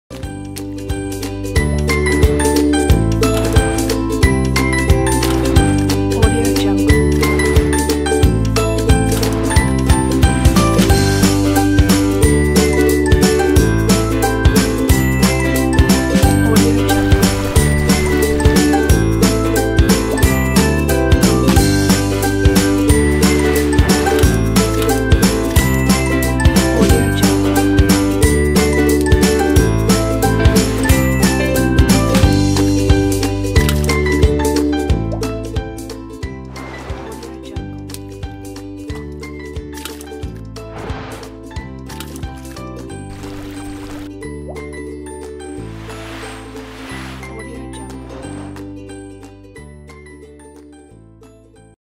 Splash Elements
Splash Elements is an amusing After Effects template composed of funny animated splashing elements and transitions. It can be easily added to your works as overlays to make the perfect way to enhance the look of a text or logo appearance, pool party videos, trip to the water park, holidays by the sea, cartoons, stories, cool slideshows and so much more. Amaze your viewers with this cartoon splash elements in your videos. Sound FX included.